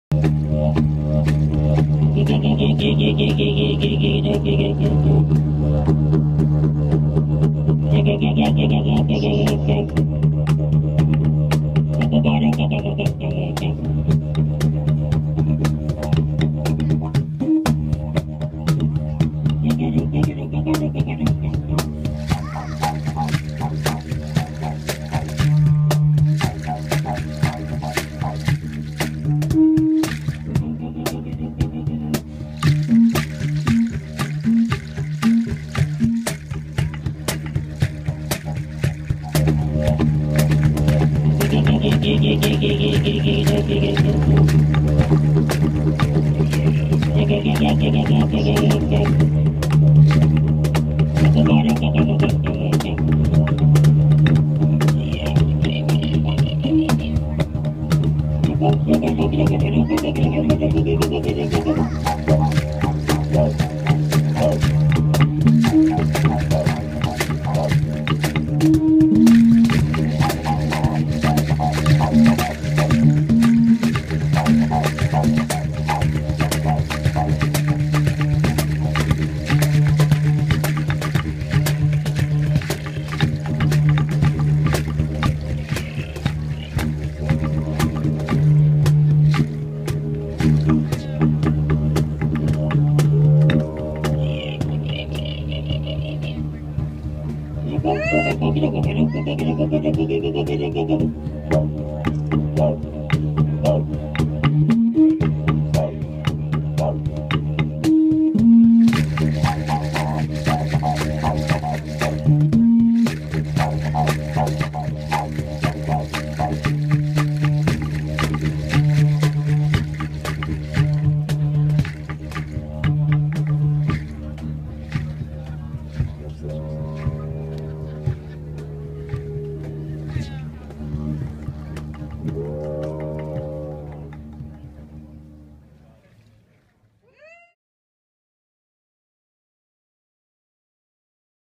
Didjeridoo 's River Light
At Dunedin on Valentine's Day with Colleen,
 Rob Thomas & Tanya Gerard -Inlakesh Music, AUdrey & Jim Rinker